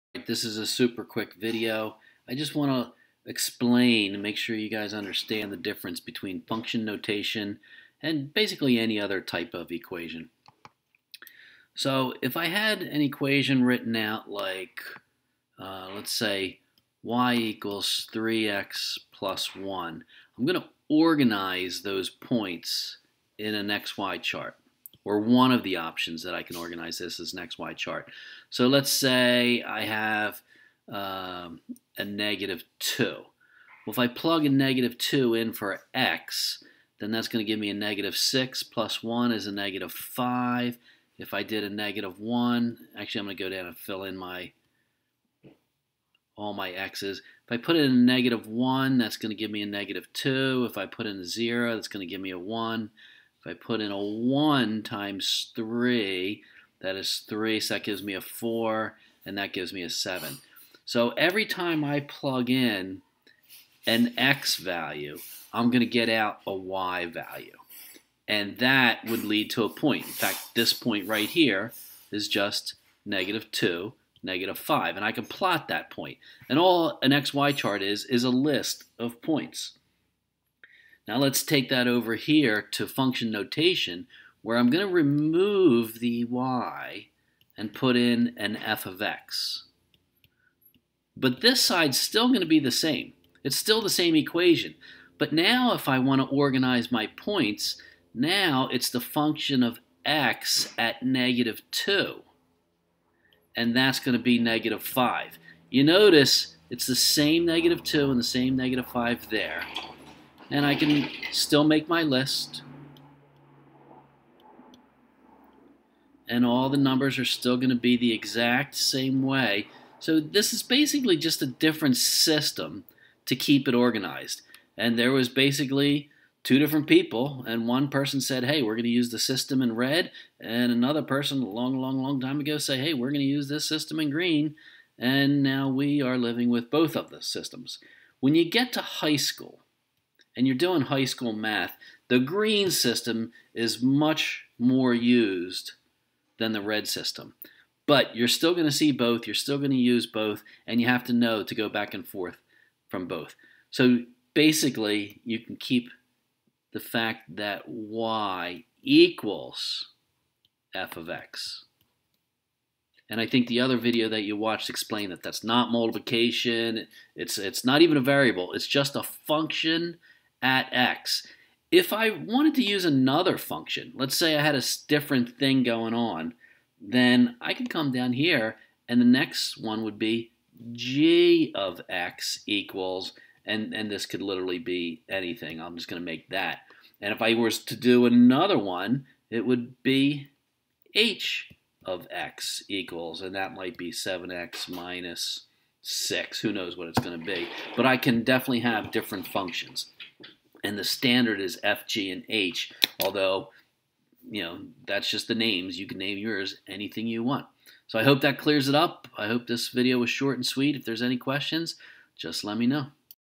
Function notation vs slope-intercept form
How do you organize points?  What is the difference between function form and slope-intercept form?  This video will help answer these questions.